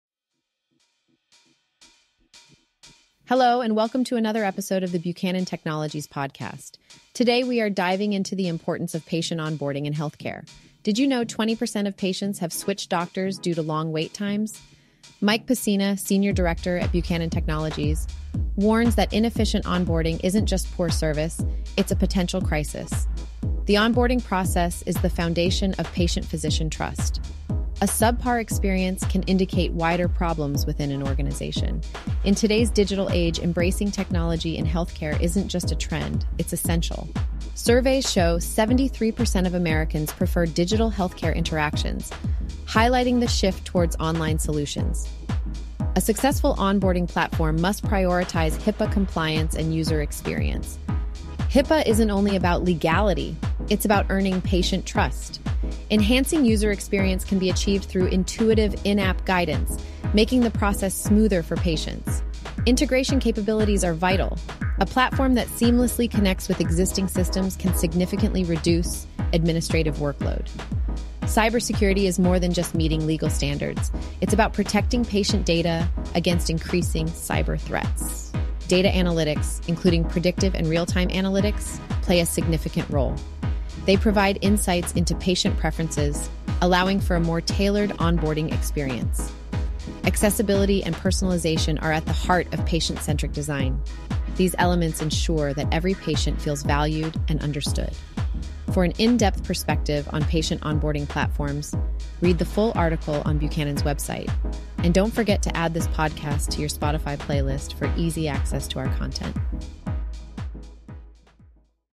Everything You Should Know About Patient Onboarding Platforms
Discover the key elements of effective patient onboarding with Buchanan Technologies. This episode delves into the importance of digital integration, personalized care, and robust security in healthcare. Watch now to understand how technology is transforming patient experiences.

Jim Buchanan - Founder and CEO